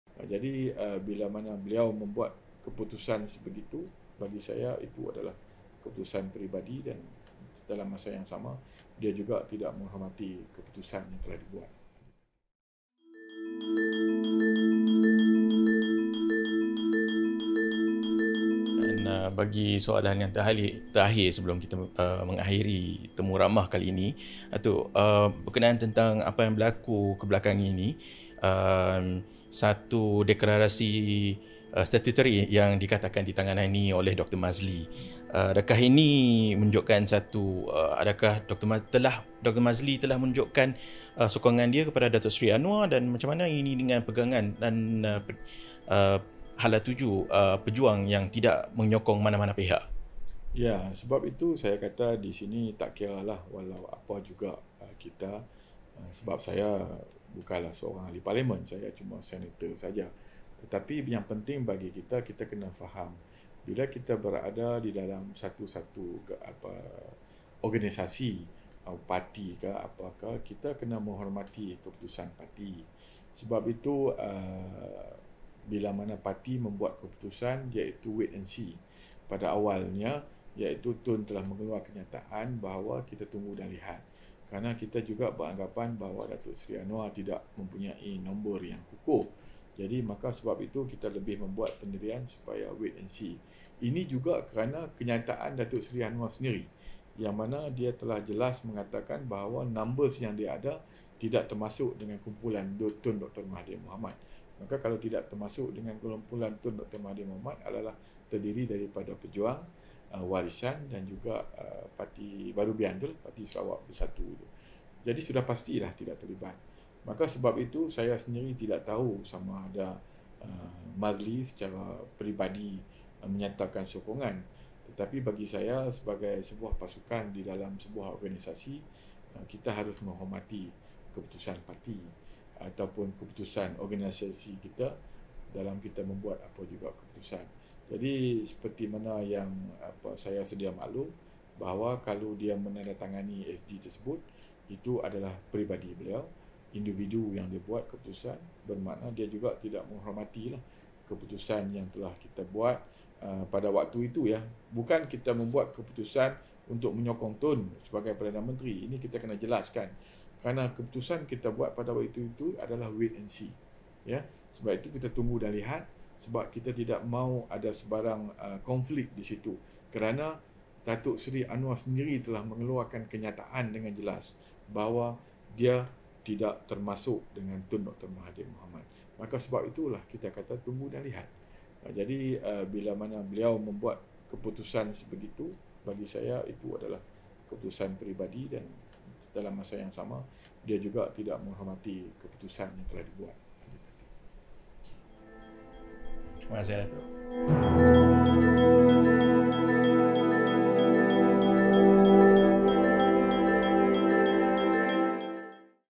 Downright disrespectful if Maszlee’s support for Anwar true: Marzuki
PUTRAJAYA – Maszlee Malik’s purported move to sign a statutory declaration (SD) in support of opposition leader Datuk Seri Anwar Ibrahim’s ambition to become prime minister is nothing short of disrespectful, said Pejuang.

Deputy president Datuk Marzuki Yahya said if this proves true, the Simpang Renggam MP’s decision does not reflect the party’s “wait and see” stand.

When Pejuang took its position based on chairman Tun Dr Mahathir Mohamad’s directive, “we assumed that Anwar did not have a strong number. That was why we made that stand”, Marzuki told The Vibes in an interview at his office here.

Interviewed and Produced by Norman Goh